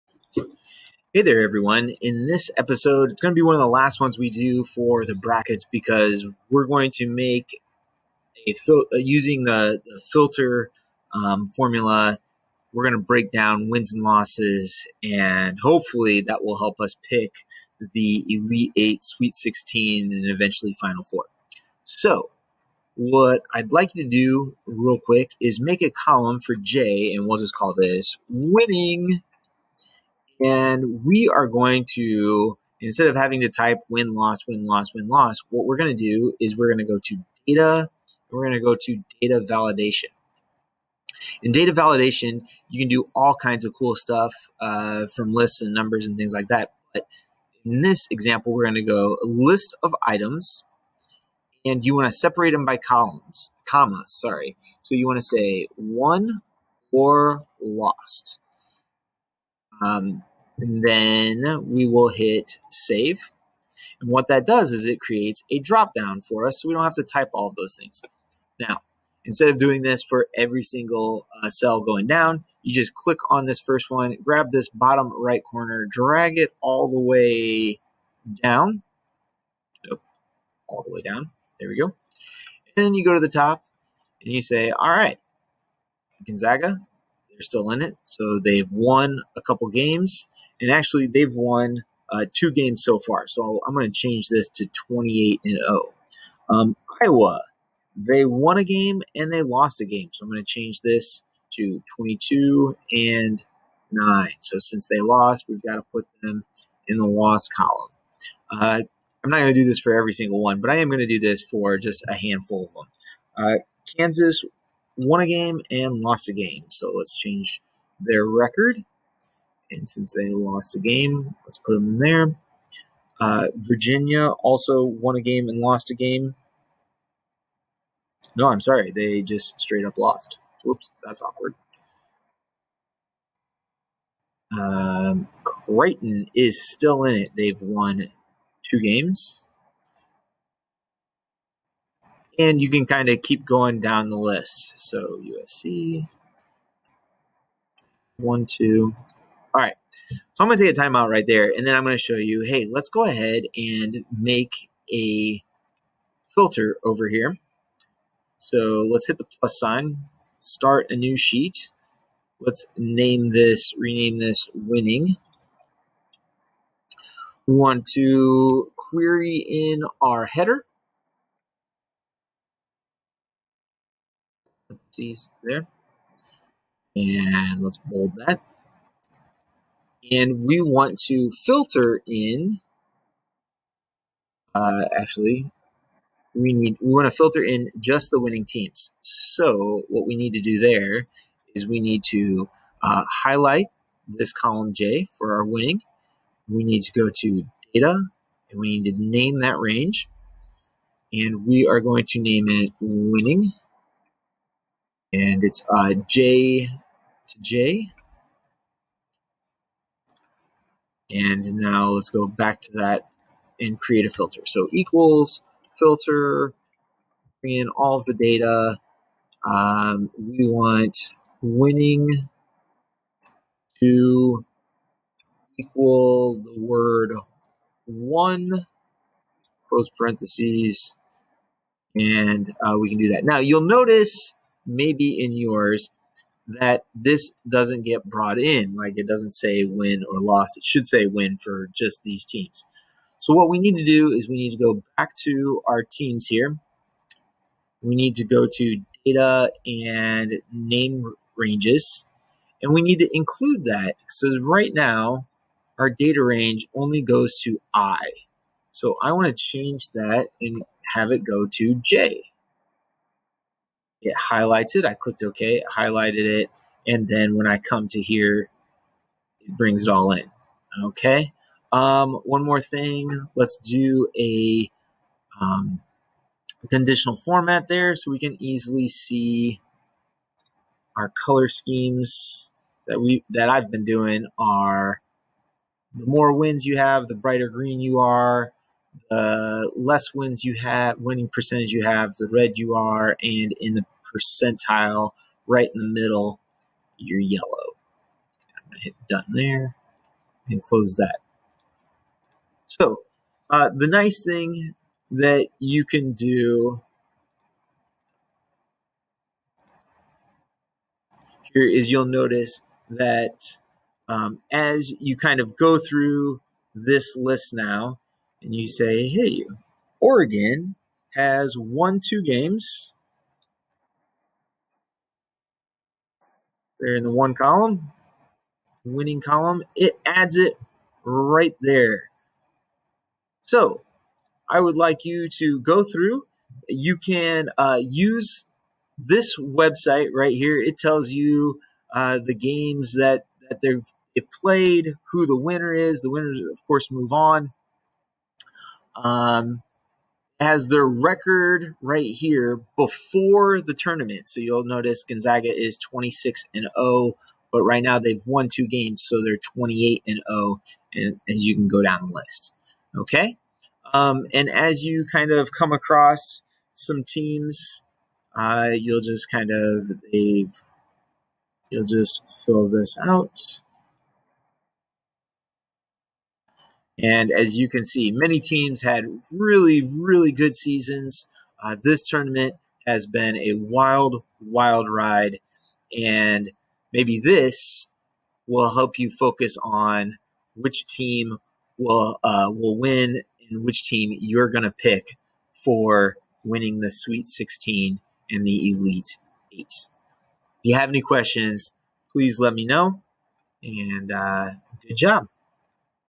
Data Validation & Filter in Google Sheets
March Madness is one of my favorite ways to use Google Sheets in class. In this tutorial, I show you how to format your data and use conditional formatting, create a data validation drop-down, and help pick the winners.  We also bold, center, and lock the header. We use query and filter formulas to separate a large group of data into smaller sets of data.